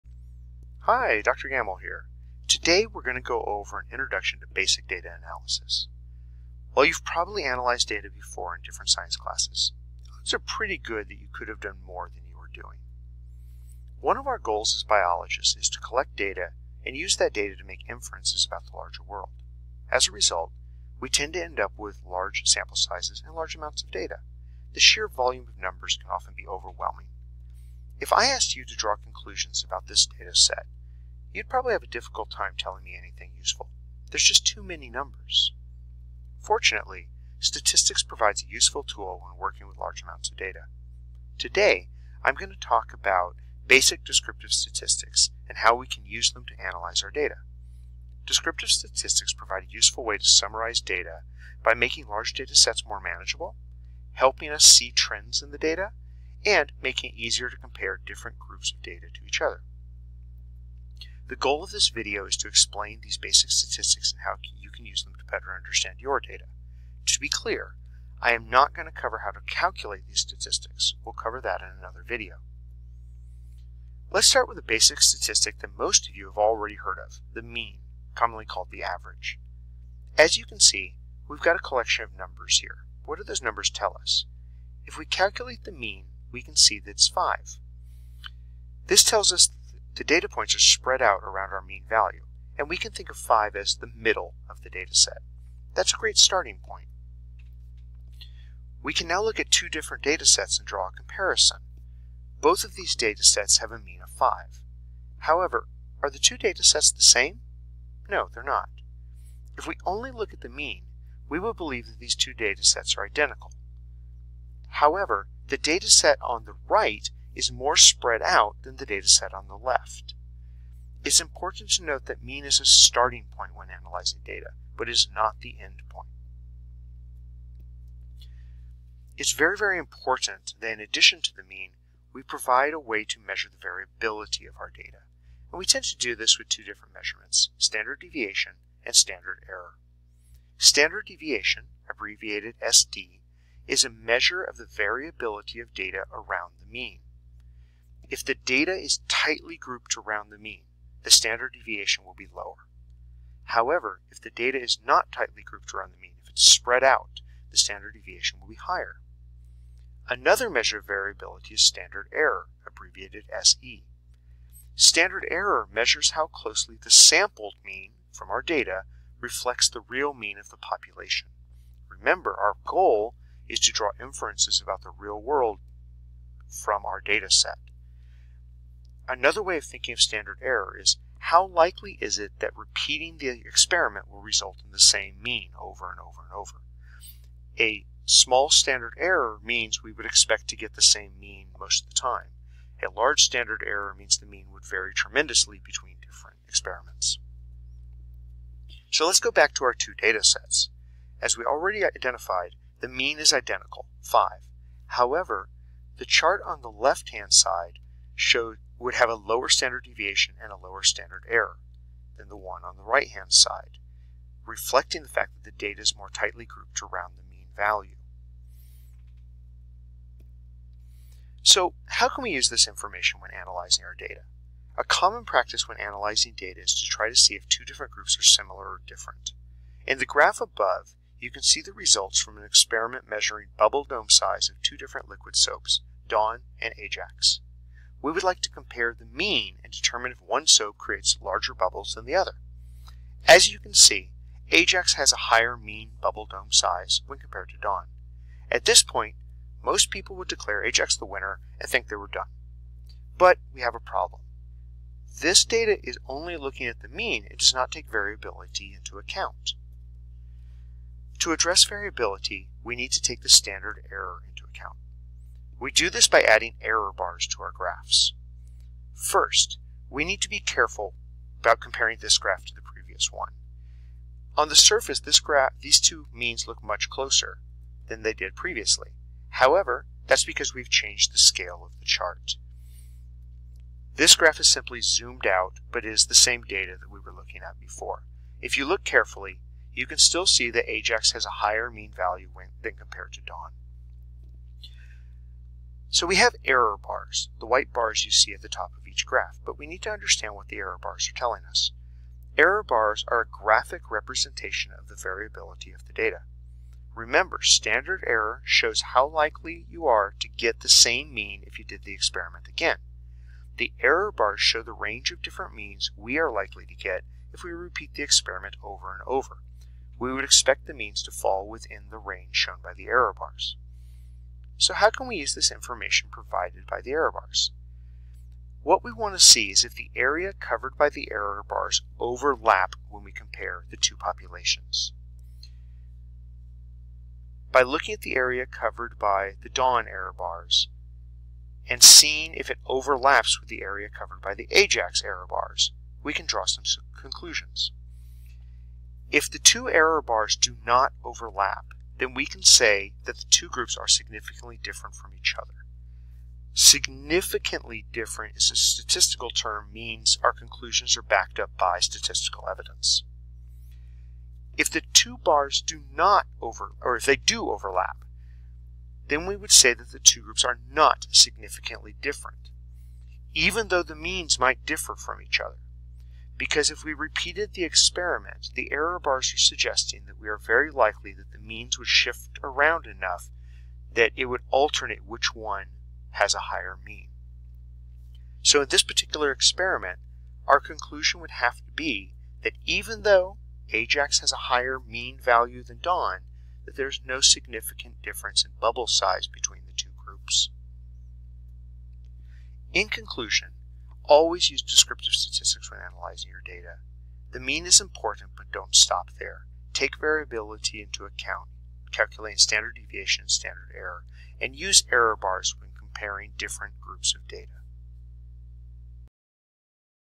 Intro to Data Analysis
A quick introduction for high school students into data analysis using descriptive statistics (mean, standard deviation, and standard error).